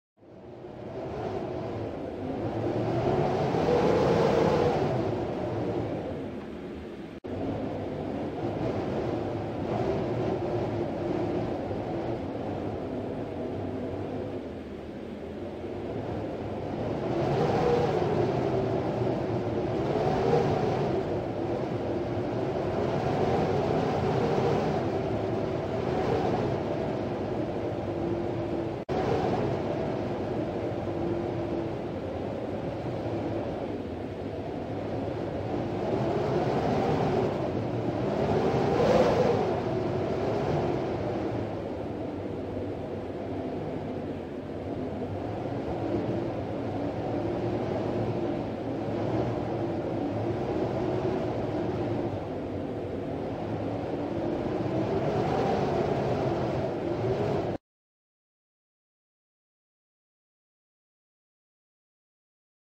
Day 8: Gathered In
A chicken in a hurricane? I'm sure many a feather fowl have endured such a scenario before. This is one of 30 short films being produced to support the fundraiser over